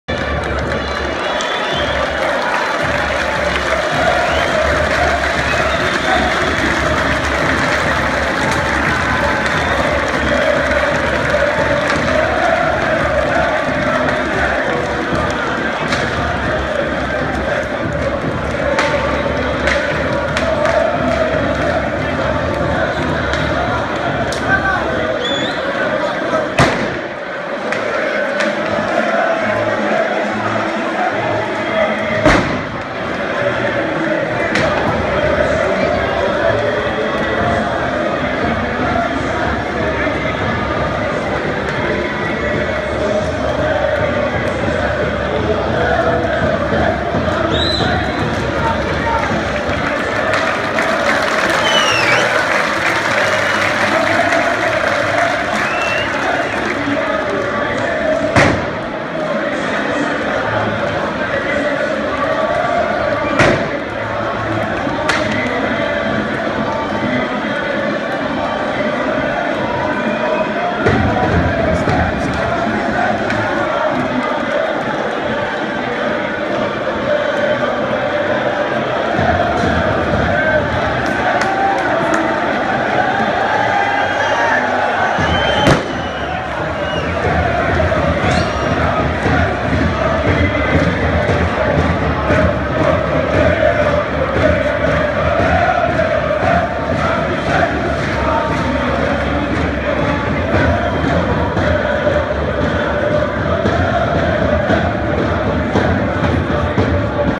ΟΦΗ - Άρης 1-1
ΟΦΗ - Άρης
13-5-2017
Η είσοδος των ομαδων και η εντυπωσιακή θύρα 4